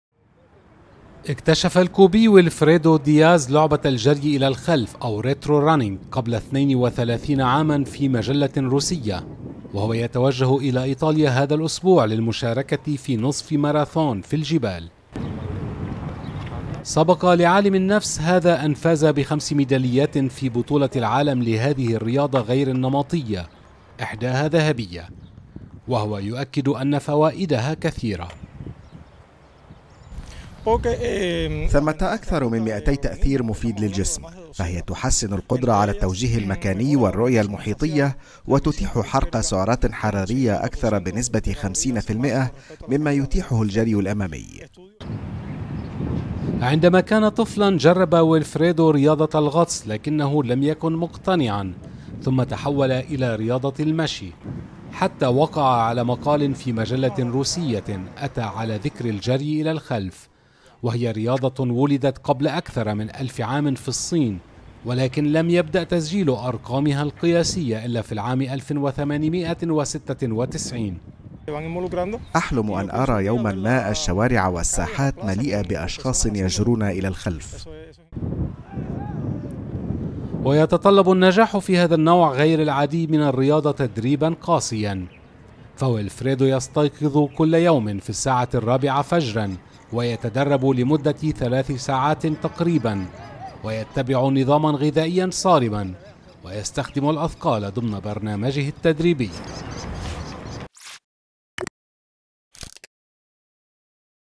عداء كوبي يتحدى الجبل بالجري إلى الخلف
اكتشف الكوبي ويلفريدو دياز لعبة الجري إلى الخلف أو "ريترو رانيننغ" قبل 32 عاماً في مجلة روسية، ويتوجه إلى إيطاليا هذا الأسبوع للمشاركة في نصف ماراثون ... في الجبال.